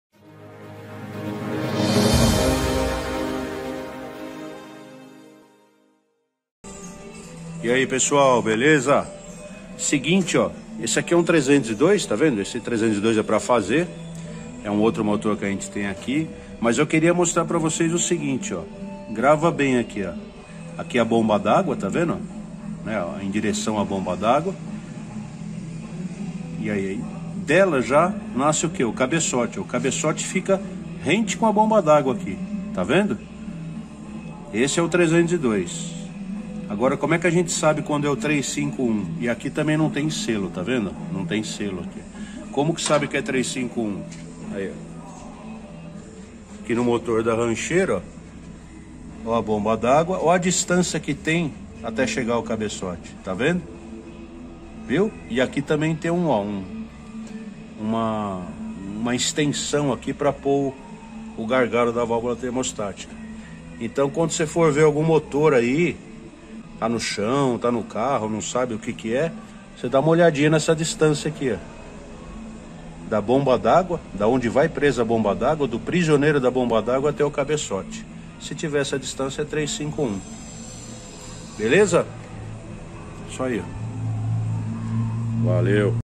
MOTORES FORD V8 302 E 351 WINDSOR - VOCÊ SABE DISTINGUIR UM DO OUTRO? É SÓ ASSISTIR ESSE VÍDEO
Diz a lenda que certa vez um Maverick famoso, que ganhava quase todas em interlagos, estava correndo como o 351 ao invés do 302, que era o regulamentado 😋

E quem descobriu foi um preparador experiente que, só de bater os olhos, descobriu a "maracutaia" 😁😁😁

Então, se você quer aprender a distinguir um motor 351 de um 302, assiste esse vídeo.

Você vai ver que é bem fácil 👍

Ah, ia me esquecendo... O método G34 que ensina a montagem do 302 pode e deve ser aplicado na montagem do 351.

Então, coloque a mão na graxa e cuide você mesmo do seu Ford V8...

Torne-se um Hot Rodder!

Valeu!